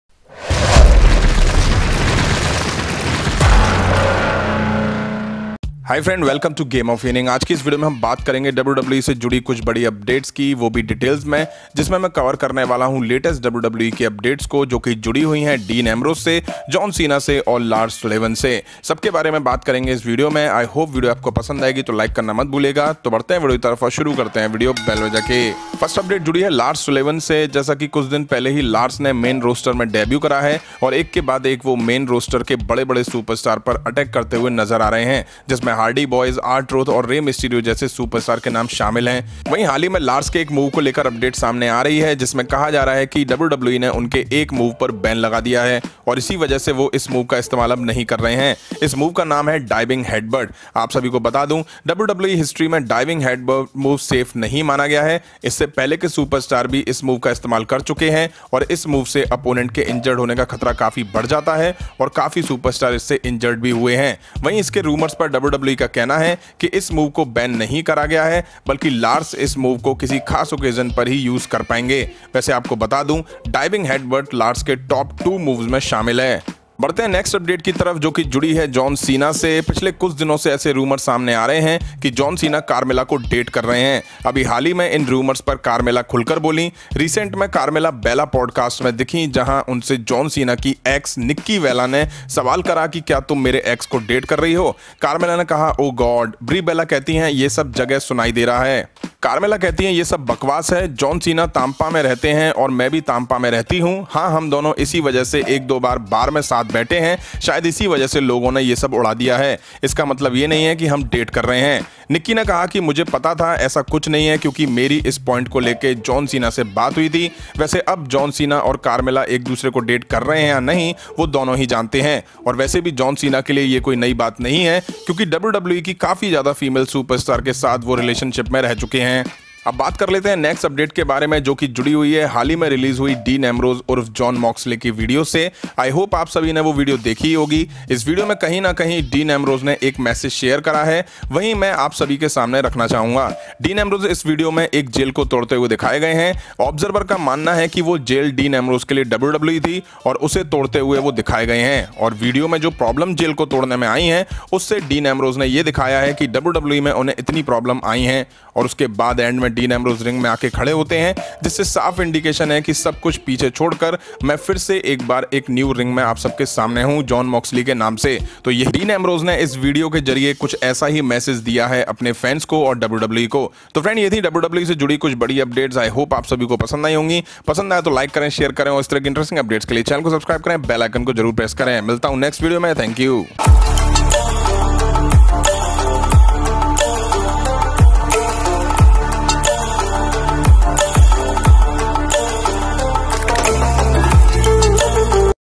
What Message in Dean Ambrose Video ! John Cena Dated Carmella ! Lars Sullivan Move Banned ?
This video is about wwe update May 3rd, 2019. Here I Cover Some interesting update about wwe, all udpate topic given below

1- What is the Coclusion of Dean Ambrose Video.
2- John Cena Dated Carmella
3- WWE Banned Lars Sullivan Move
4- Carmella React on John Cena Relationship
5- What is the Actual Meaning of Dean Ambrose Video Jon Moxley